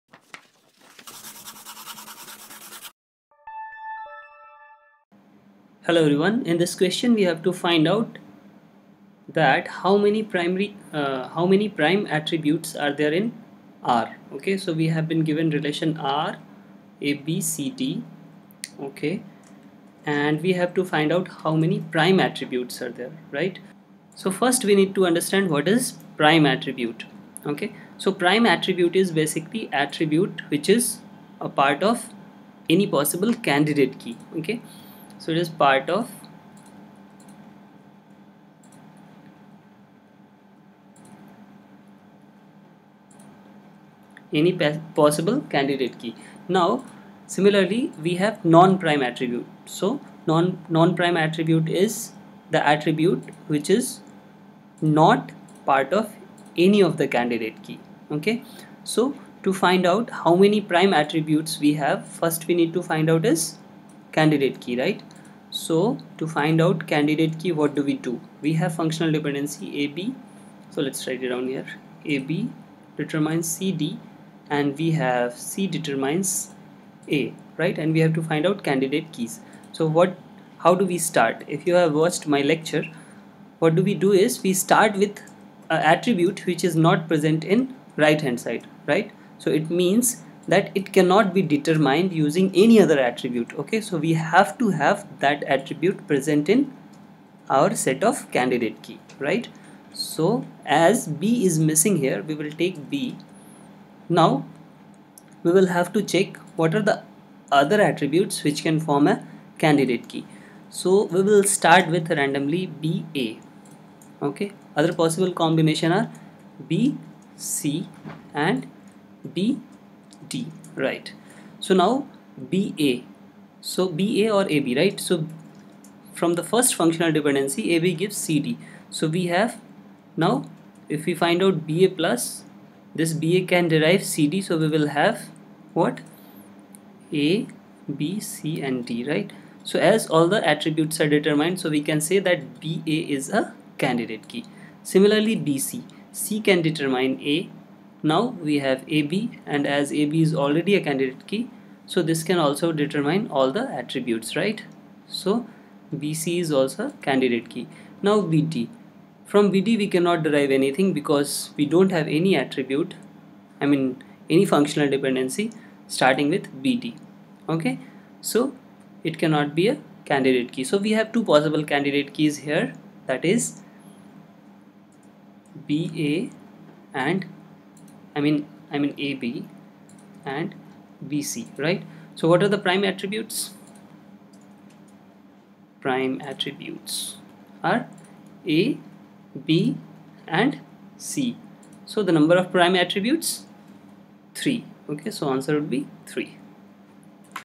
Let R(ABCD) | F = {AB → CD, C → A}. How many prime attributes are there in R?
Let R(ABCD) be a relation schema with FD set F = {AB → CD, C → A}. How many prime attributes are there in R?